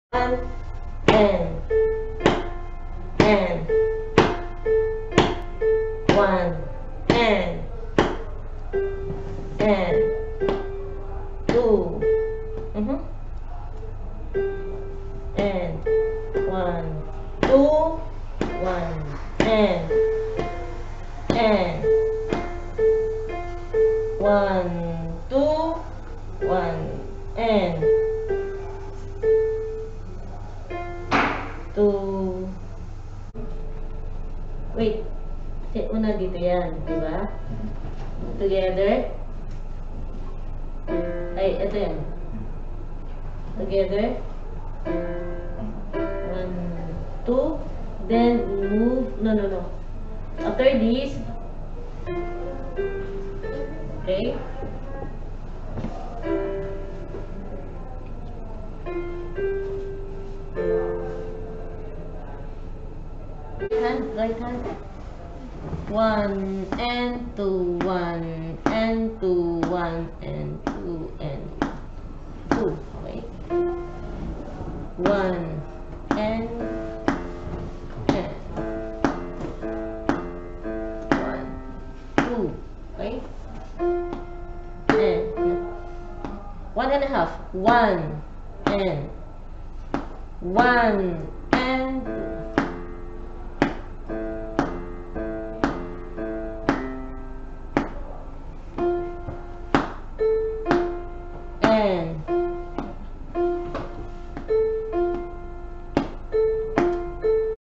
Maharlika Academy- Casa De Arte ( Piano Class with Teacher Lilian)
MA Homeschool under Casa De Arte Piano Class with Abdurahman (Grd10), Ahmad (Grd7), and Fatima Bello Hussin(Grd6).